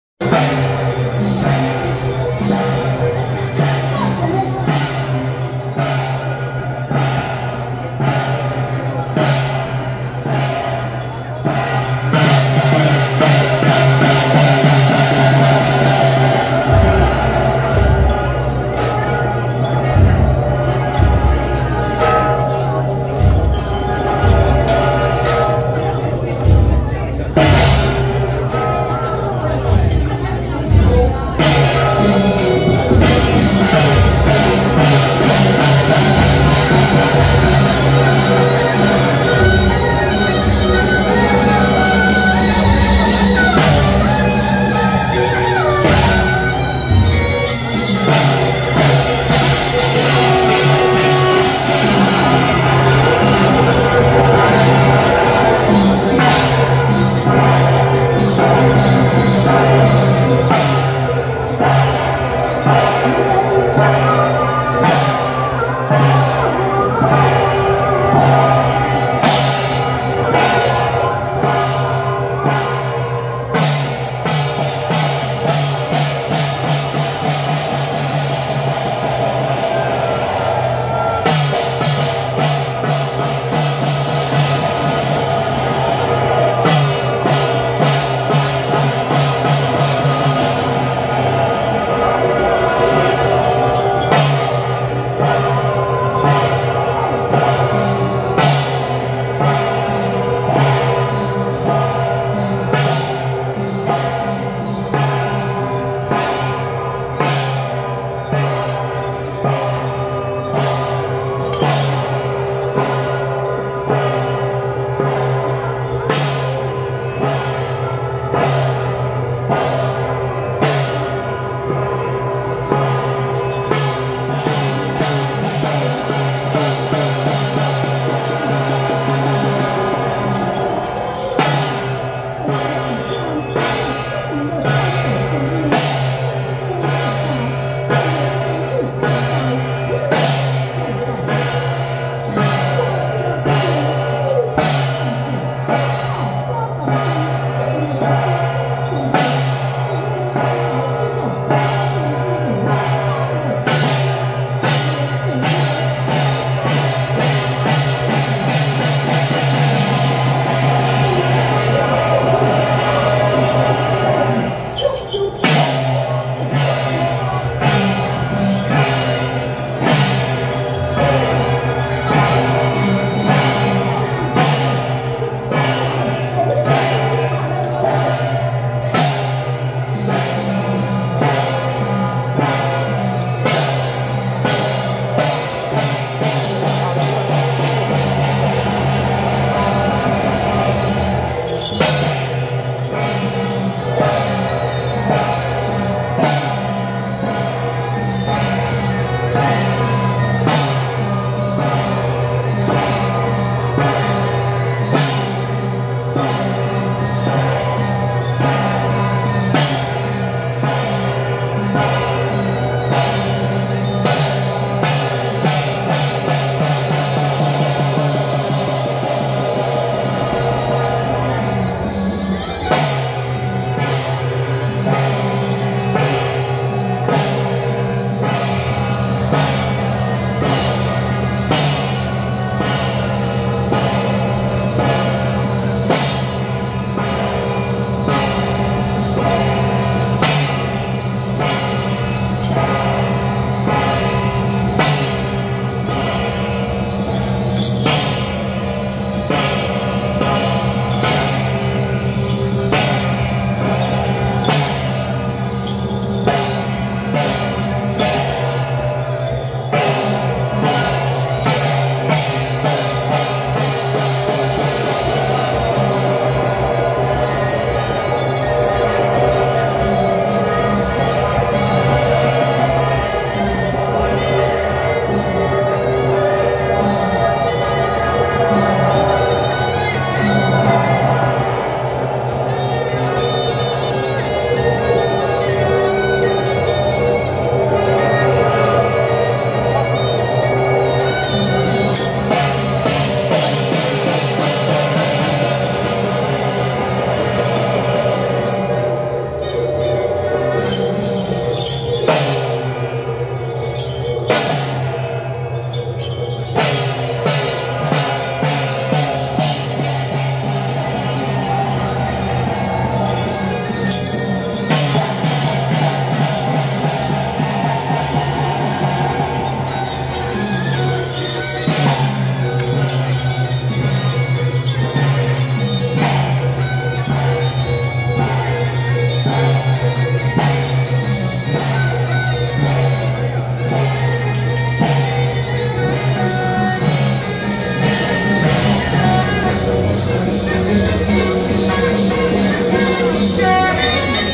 【艋舺堀江町-官將首】~112萬里昭靈宮恭迎關渡二媽遶境
艋舺堀江町-官將首